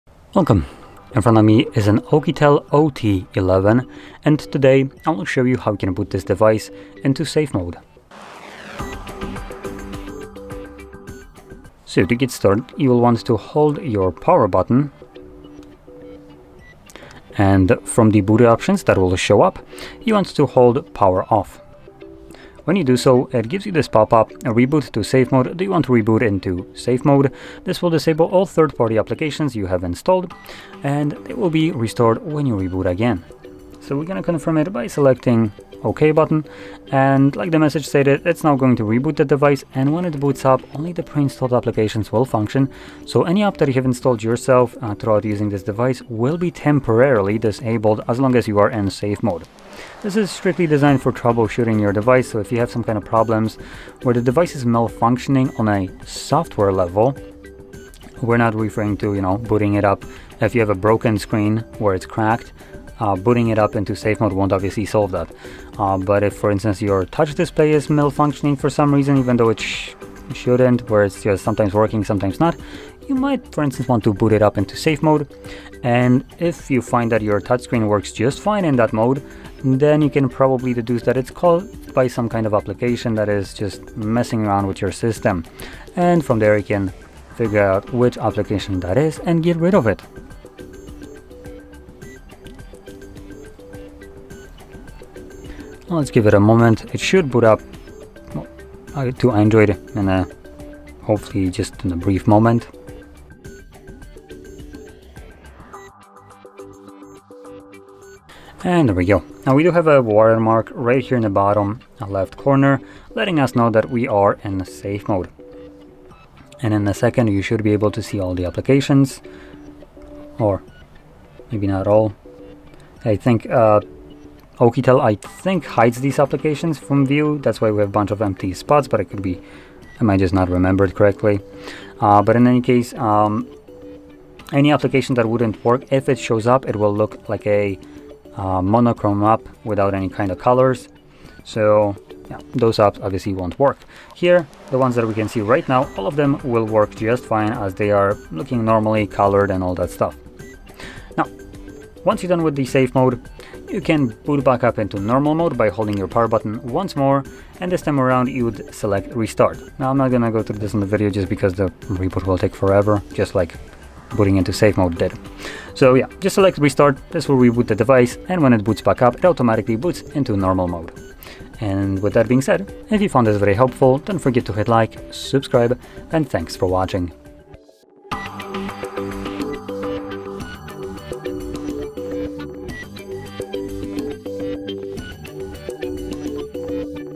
How to Boot OUKITEL OT11 into Safe Mode - Start Your Device in Safe Mode
If you’re encountering issues with your OUKITEL OT11 and need to troubleshoot, booting into Safe Mode can help. In this video, we’ll show you how to start your device in Safe Mode, which can assist in identifying and resolving problems. So, if you want to learn how to enter Safe Mode, let's get started!

How to Boot OUKITEL OT11 into Safe Mode? How to Start Safe Mode on OUKITEL OT11? How to Use Safe Mode on OUKITEL OT11?